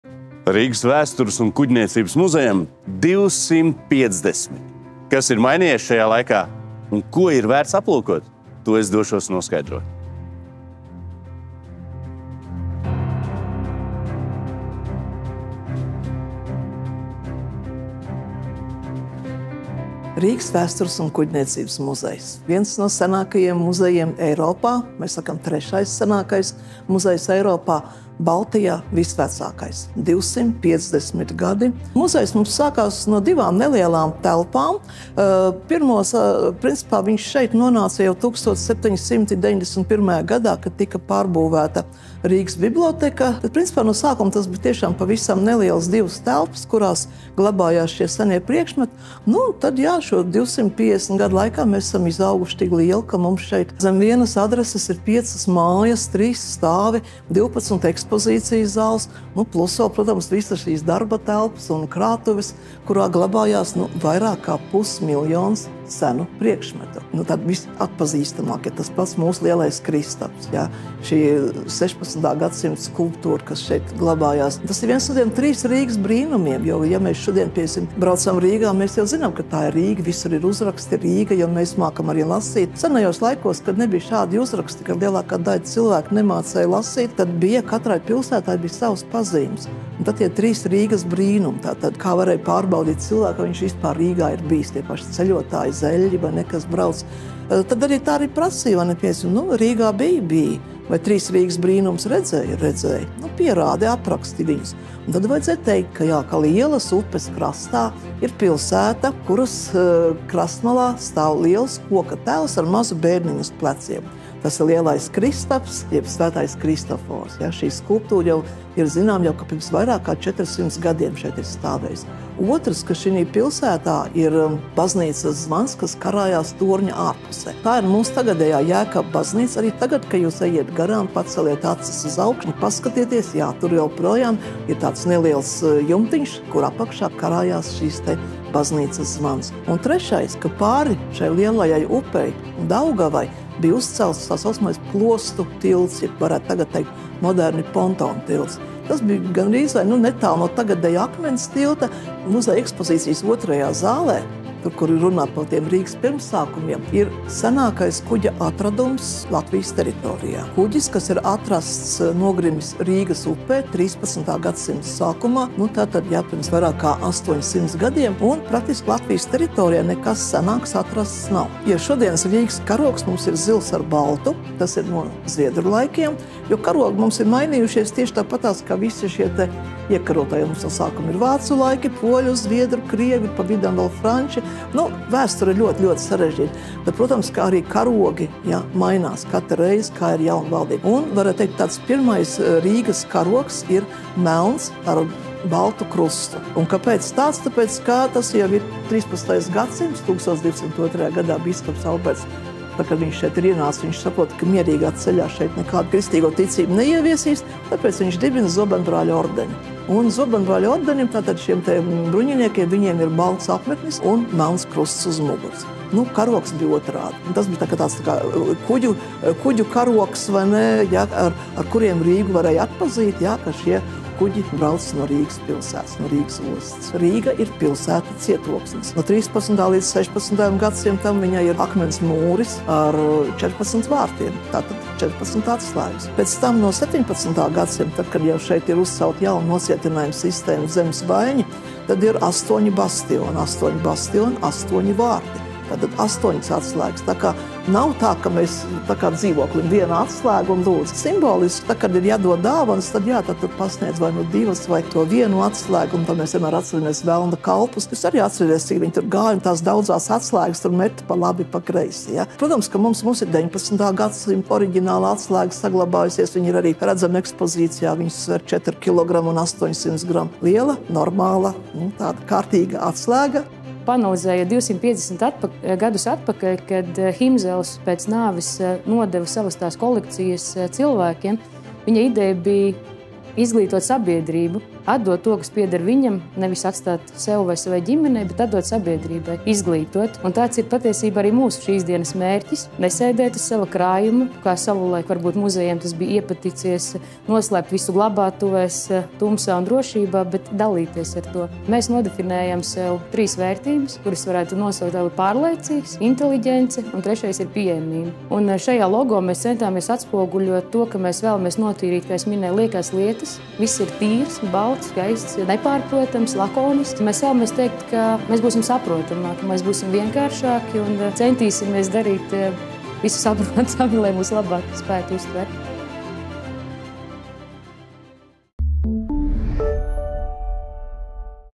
Baltijas vecākais muzejs atzīmē 250 gadu jubileju
Rīgas vēstures un kuģniecības muzejs ir trešais vecākais Eiropā un vissenākais savas jomas pārstāvis Baltijas valstīs. Šogad tam aprit apaļi 250 gadi.

Vēsturniece Irēna Strēle atklāj, ka muzeja pirmsākumi pirms vairāk nekā diviem gadsimtiem bijušas divas nelielas istabiņas, bet šajā iespaidīgajā laika posmā tas piedzīvojis vērienīgu izaugsmi – līdz 12 ekspozīciju telpām, vairākām ēkām, darba telpām un muzeja krātuvēm, kurās glabājas vairāk nekā pusmiljons senu priekšmetu. Muzeja ekspozīcijā apskatāmi patiesi unikāli priekšmeti, bet aizraujošākā ir galvaspilsētas vēsture – Rīgā mainījušās varas un karogi, norisinājušās kaujas un valdījusi aktīva kultūras dzīve. Skaties video un uzzini par trīs Rīgas brīnumiem un melno karogu, kā arī un citus aizraujošus faktus par Latvijas galvaspilsētu.